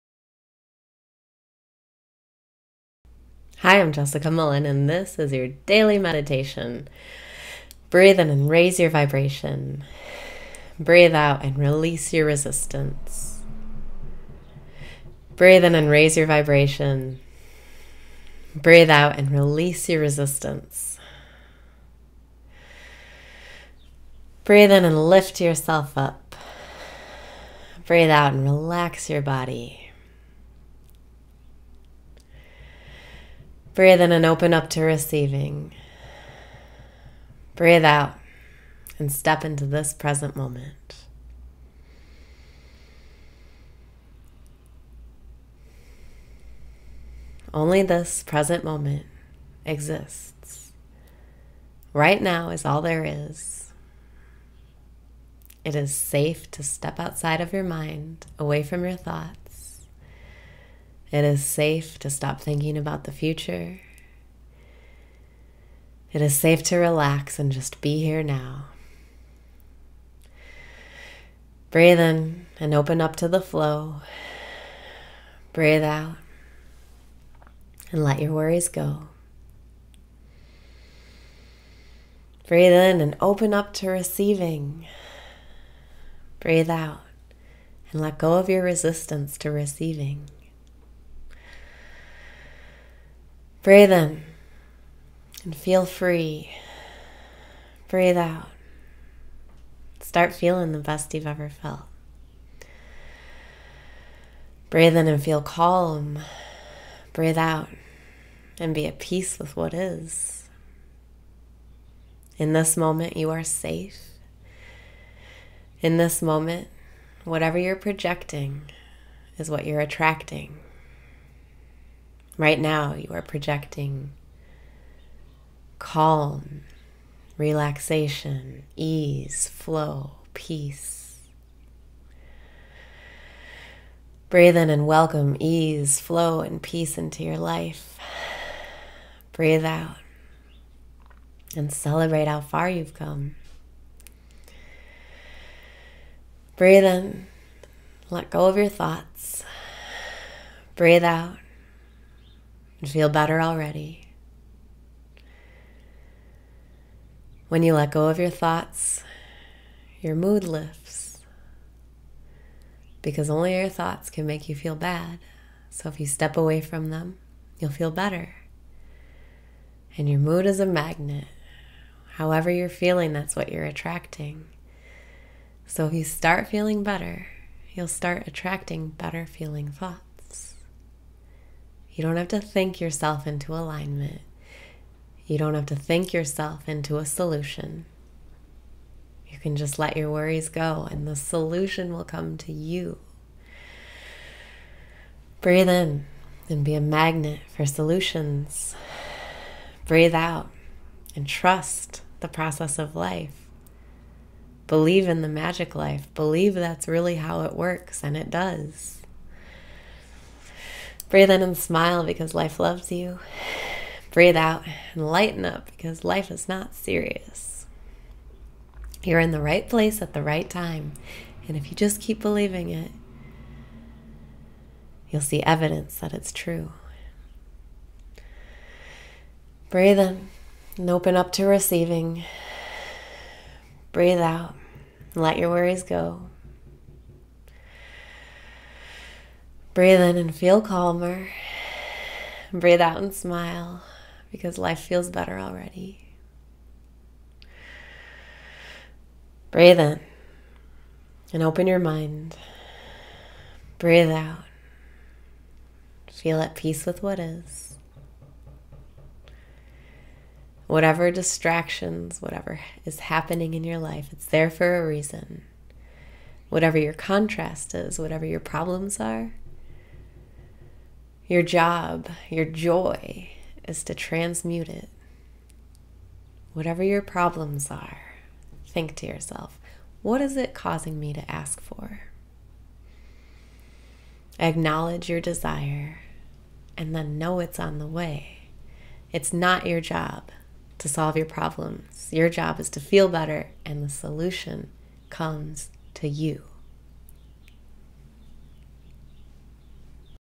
Daily Meditation - The Solution Comes to You
Breathe out and step into this present moment

Only this present moment exists. Right now is all there is. It is safe to step outside of your mind, away from your thoughts. It is safe to stop thinking about the future. It is safe to relax and just be here now.

Breathe in and open up to the flow

Breathe out and let go of your resistance to receiving

Breathe in and feel free!
Breathe out and start feeling the best you’ve ever felt!

Breathe in and feel calm
Breathe out and be at peace with what is

In this moment, you are safe. In this moment, whatever you’re projecting is what you’re attracting. Right now you are projecting calm, relaxation, ease, flow, peace.

Breathe in and welcome ease, flow and peace into your life

Breathe in and let go of your thoughts
Breathe out and feel better already

When you let go of your thoughts, your mood lifts, because only your thoughts can make you feel bad. If you step away from them, you’ll feel better. Your mood is a magnet—however you’re feeling, that’s what you’re attracting. If you start feeling better, you’ll start attracting better-feeling thoughts. You don’t have to think yourself into alignment .You don’t have to think yourself into a solution. You can just let your worries go and the solution will come to you.

Breathe in and be a magnet for solutions

Believe in the magick life! Believe that’s really how it works and it does!

Breathe in and smile because life loves you!
Breathe out and lighten up because life is not serious!

You’re in the right place at the right time and if you just keep believing it, you’ll see evidence that it’s true.

Breathe in and feel calmer
Breathe out and smile because life feels better already

Breathe in and open your mind
Breathe out and feel at peace with what is

Whatever distractions, whatever is happening in your life—it’s there for a reason. Whatever your contrast is, whatever your problems are—your job, your JOY—is to transmute it. Whatever your problems are, think to yourself, “What is it causing me to ask for?” Acknowledge your desire then know it’s on the way. It’s not your job to solve your problems. Your job is to feel better, and the solution comes to you!